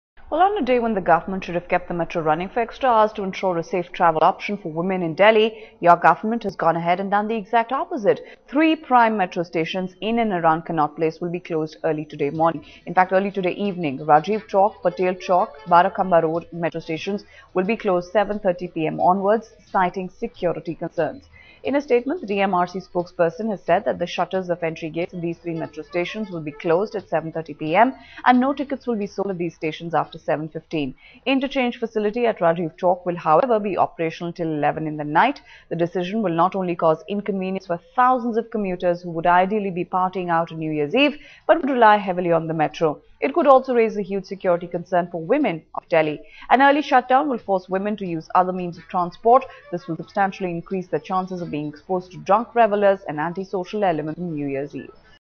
Three Delhi Metro stations around Connaught Place to close early on New Year's eve
Rajiv Chowk, Barakhamba Road and Patel Chowk Metro stations will be closed from 7:30 pm on New Year's eve. However inter-change will be allowed at Rajiv Chowk station.